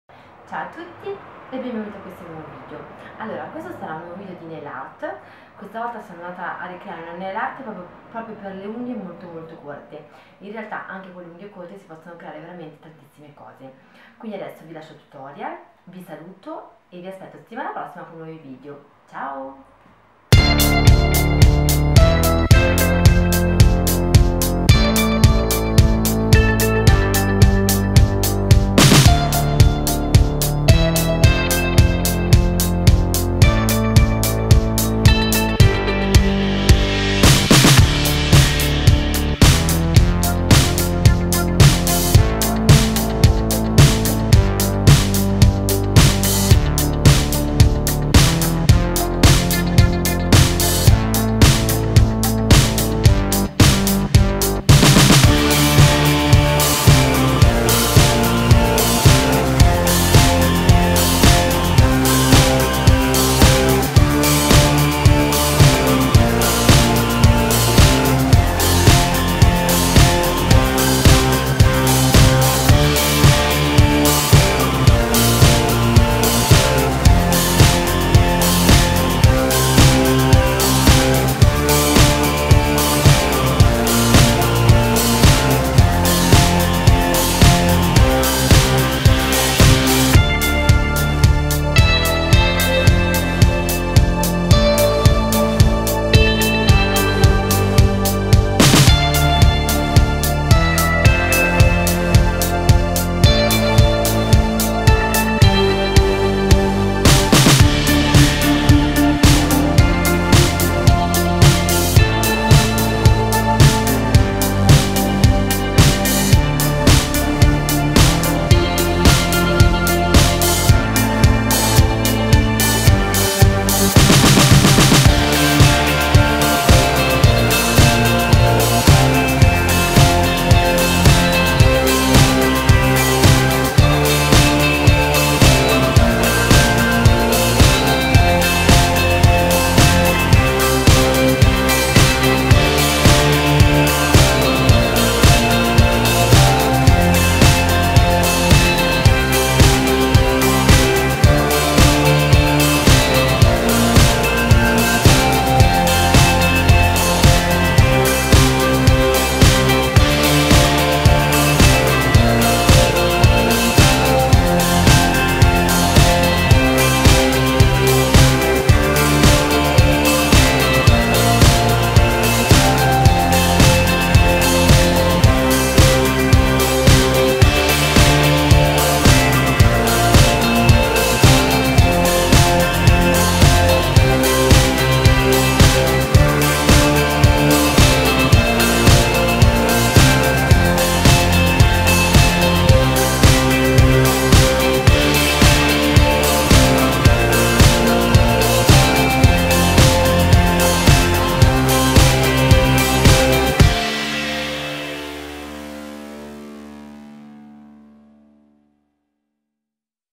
Nail Art per unghie corte
Ciao a tutti .in questo video vi mostro un tutorial di una Nail art ideale per le unghie corte. ^_^ alla prossima!kisses